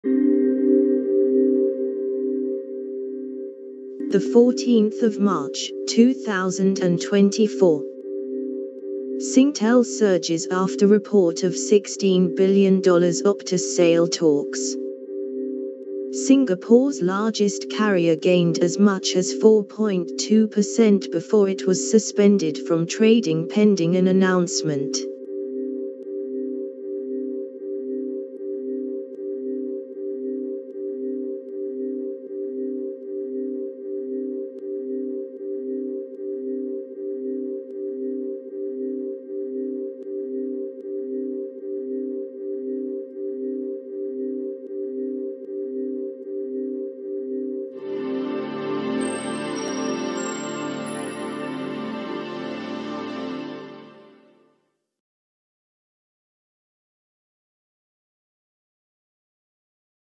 [Latest]Singtel surges after report of $16 billion Optus sale talks
[Contents]
Singapore’s largest carrier gained as much as 4.2 per cent before it was suspended from trading pending an announcement.